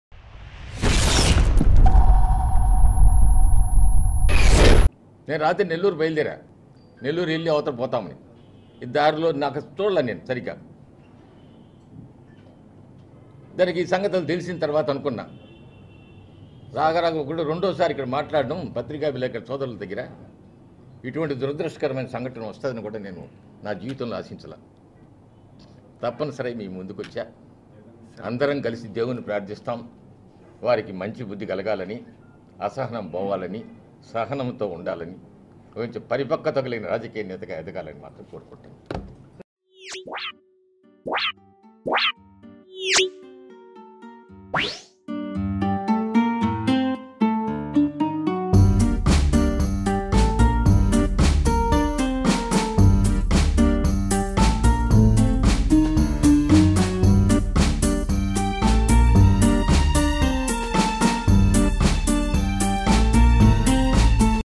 Janasena Leaders FIres on Ys Jagan Mohan Reddy | Comments Pawan Kalyan Three Marriages
Janasena Leaders FIres on Ys Jagan Mohan Reddy